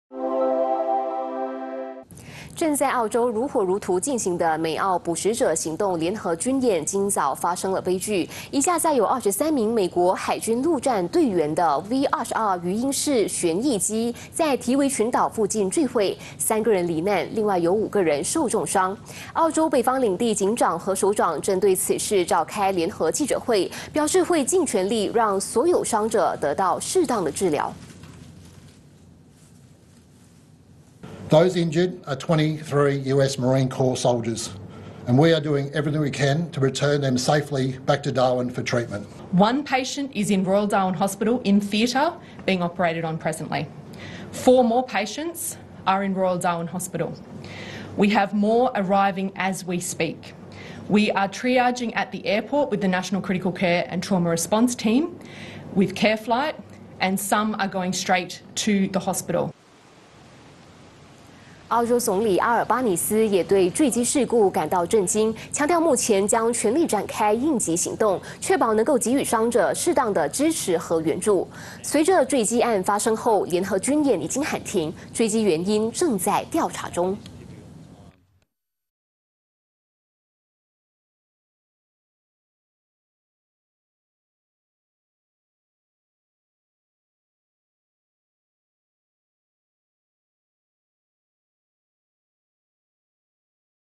美军直升机坠毁澳海域 3海军队员罹难5重伤【2023.08.27 八度空间华语新闻】
【美军直升机坠毁澳海域  3海军队员罹难5重伤】
正在澳洲如火如荼进行的“美澳捕食者行动”联合军演今早发生了悲剧。一架载有23名美国海军陆战队员的V-22鱼鹰式旋翼机在提维群岛附近坠毁，3人罹难，另有5人受重伤。

澳洲北方领地警长和首长针对此事召开联合记者会，表示会尽全力让所有伤者得到适当的治疗。澳洲总理阿尔巴尼斯也对坠机事故感到震惊，强调目前将全力展开应急行动，确保能够给予伤者适当的支持和援助。随着坠机案发生后，联合军演已经喊停。坠机原因正在调查中。

【今日主播】叶剑锋 & 刘湘灵